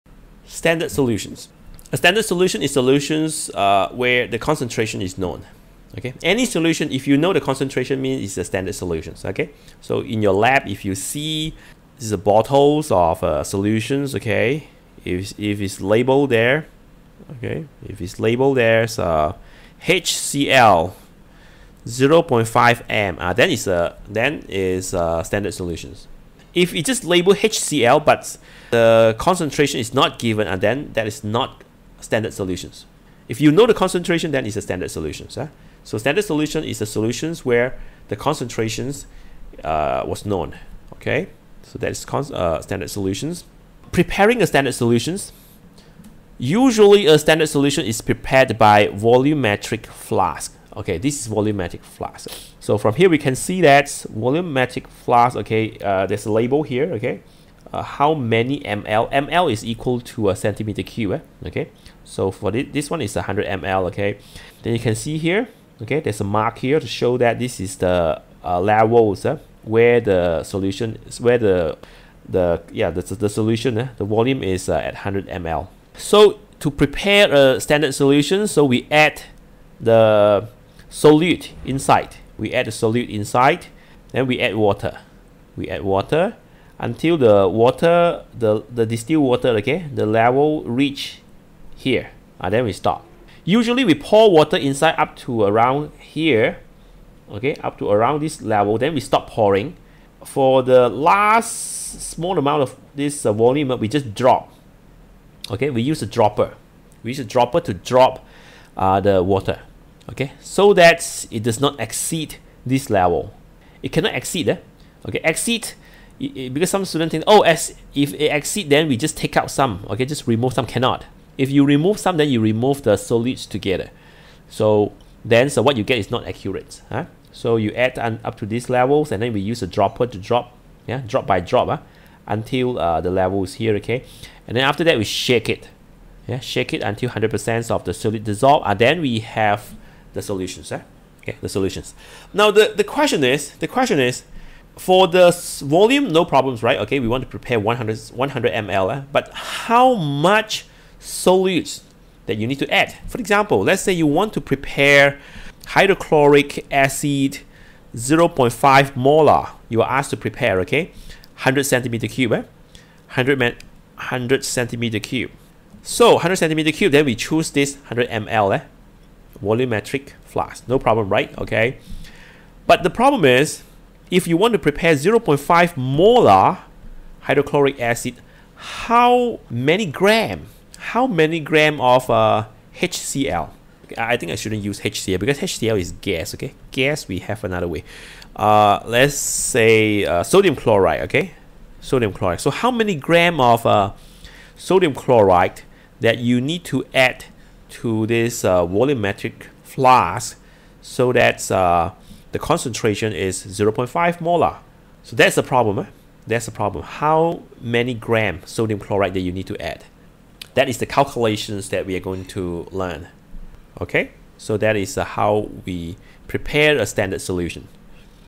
Preparing a Standard Solution | Acids and Bases
Form 4 Chemistry kssm Chapter 6 Acids, Bases and Salts

1. A standard solution is a solution whose concentration was known.
2. Usually, a standard solution is prepared by volumetric flask.
3. In the process, you need to calculate the mass of the solute and the volume of the solvent.